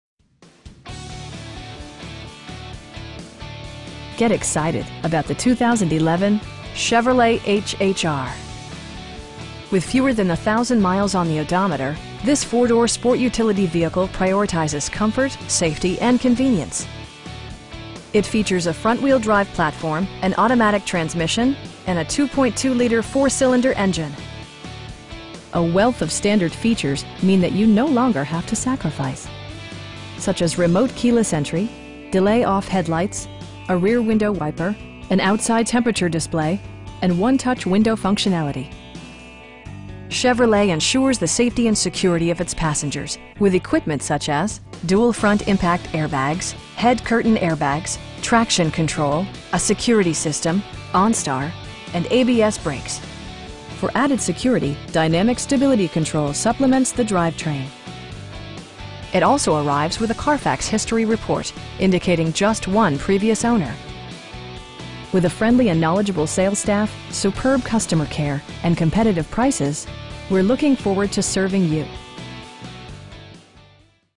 2011 Chevrolet HHR LT in Matteson, IL 60443
World Hyundai of Matteson
5337 Miller Circle Drive
in Matteson, IL 60443

Get excited about the 2011 Chevrolet HHR. With fewer than a thousand miles on the odometer, this 4 door sport utility vehicle prioritizes comfort, safety and convenience. It features a front-wheel-drive platform, an automatic transmission, and a 2.2 liter 4 cylinder engine. A wealth of standard features means that you no longer have to sacrifice. Such as remote keyless entry, delay-off headlights, a rear window wiper, an outside temperature display, front bucket seats, tilt steering wheel, cruise control, and 1-touch window functionality. Chevrolet ensures the safety and security of its passengers with equipment such as: dual front impact airbags, head curtain airbags, traction control, a security system, onStar, and ABS brakes. For added security, dynamic Stability Control supplements the drivetrain. It also arrives with a Carfax history report, indicating just one previous owner. With a friendly and knowledgeable sales staff, superb customer care, and competitive prices, we're looking forward to serving you.